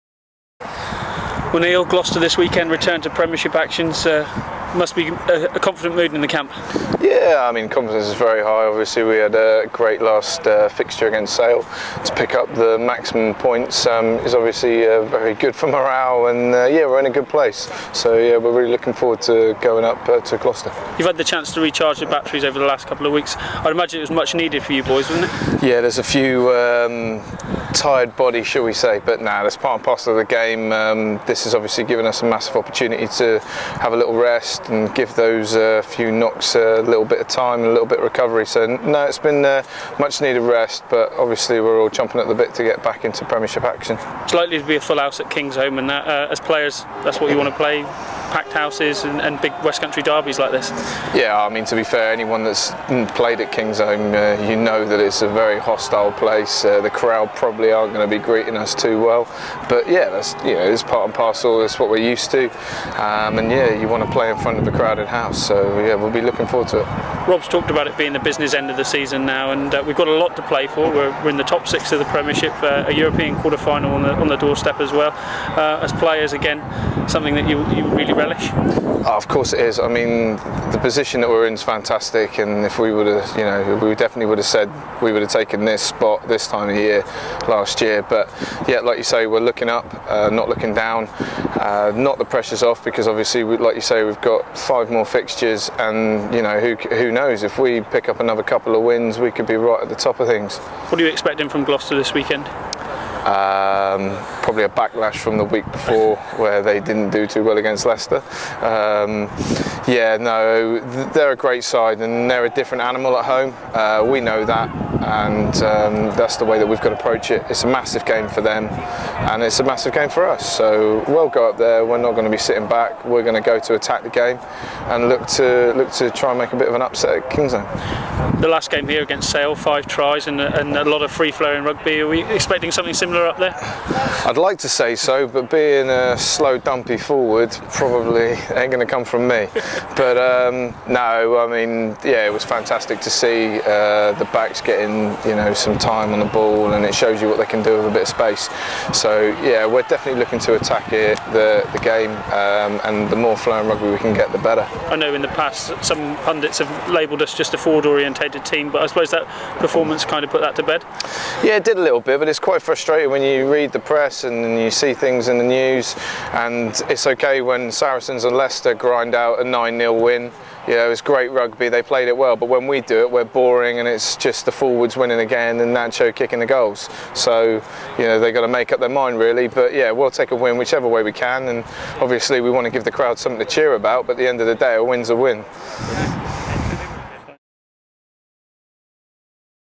Chiefs TV - Clark pre Glos.m4v
Chiefs TV - Neil Clark talks after the 2 week break in the Aviva Premiership and of the challenge of playing Gloucester this weekend...